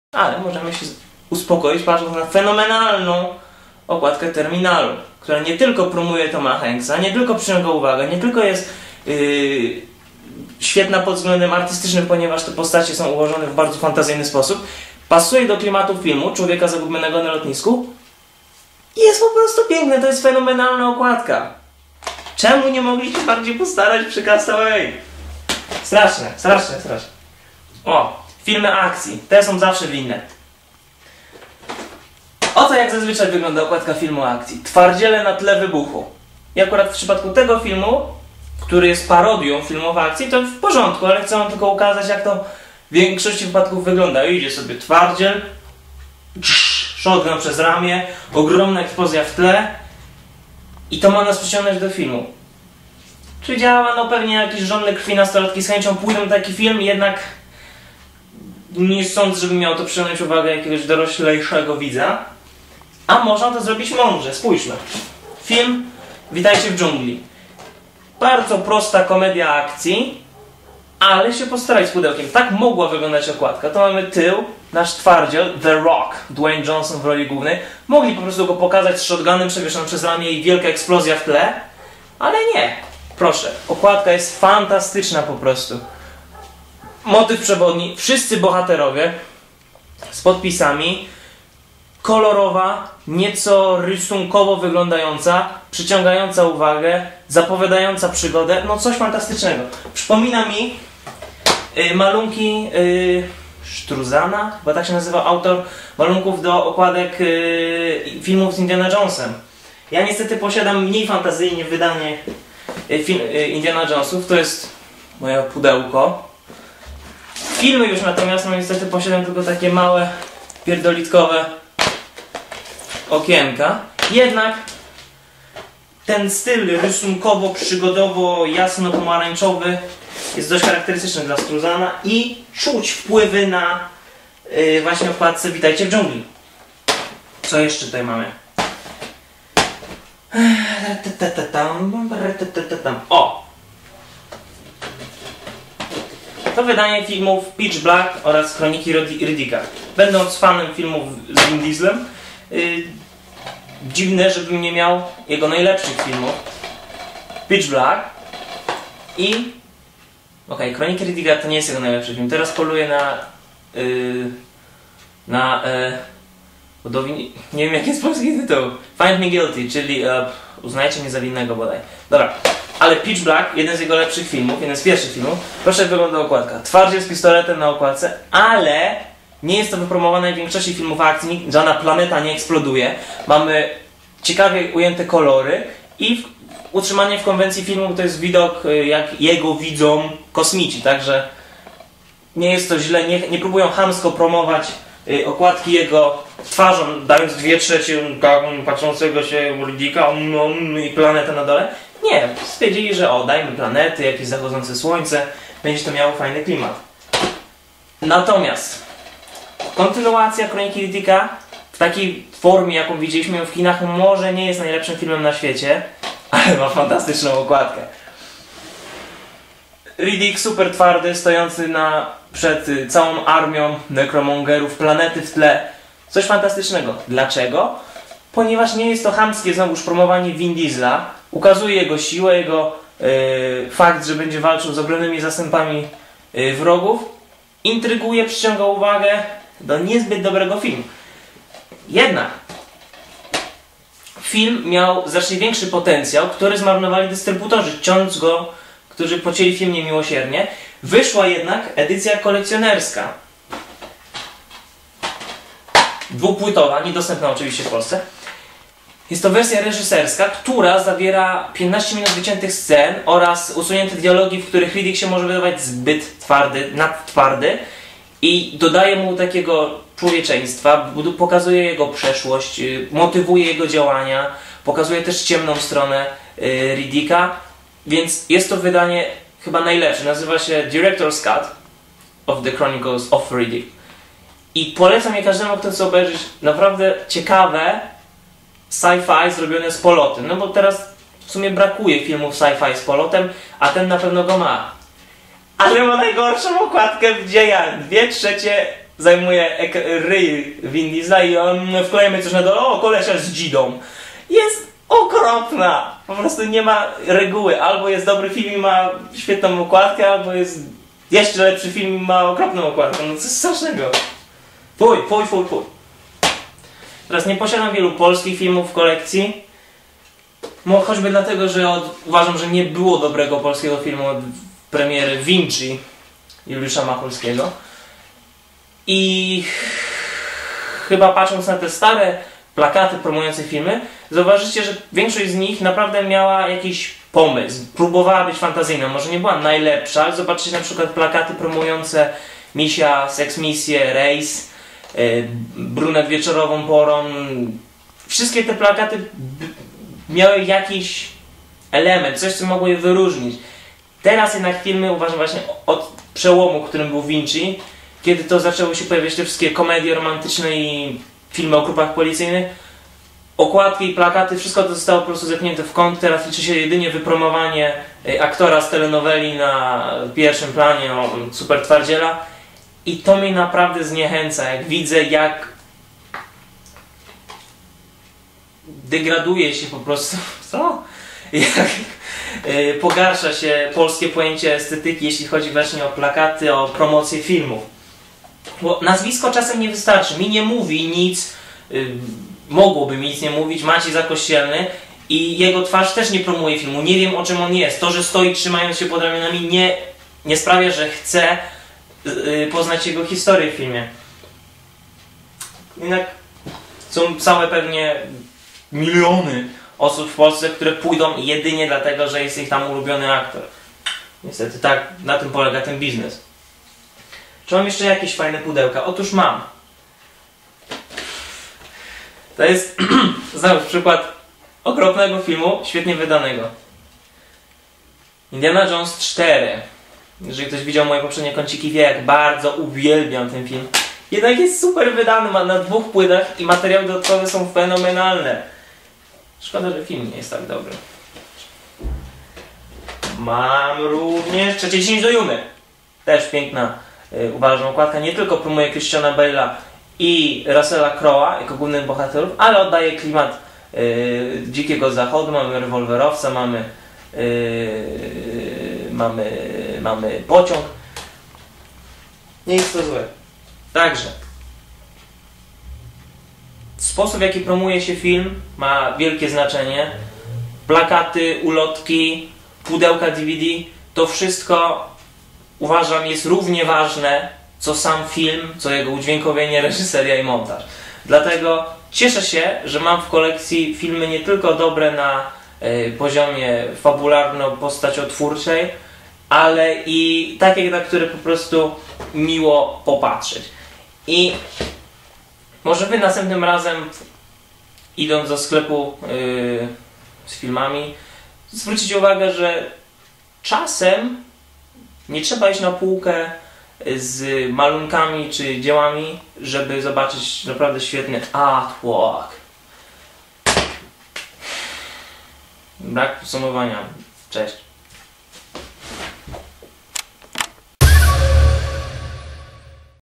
Blady kącik filmowy: Pudełka 2
Przedstawiam kolejne pudełka z mojej kolekcji, oraz kończę kącik w najmniej profesjonalny sposób.